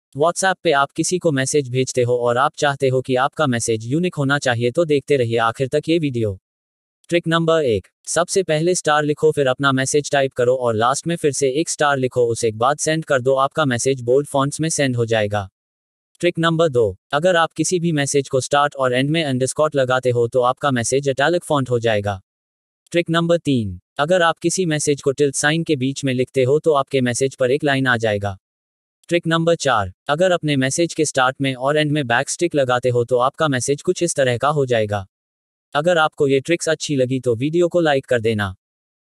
WhatsApp Tricks 2022
In this video, I'm going to share with you some of the WhatsApp tips and tricks that will save you time and make your life easier. You will learn how to create a group, set a timer for chat messages, delete chats, block contacts and much more.